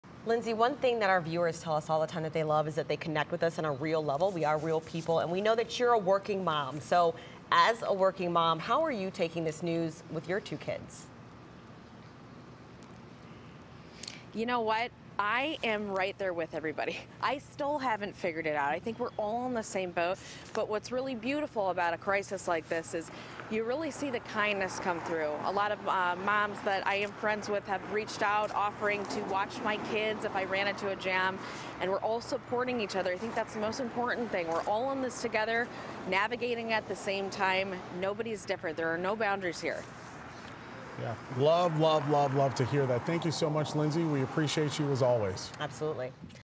3News' Lindsay Buckingham on dealing with childcare during coronavirus pandemic
Lindsay is a proud working mom, and admits she doesn't even know what's next. However, as she stated, "We're all in this together."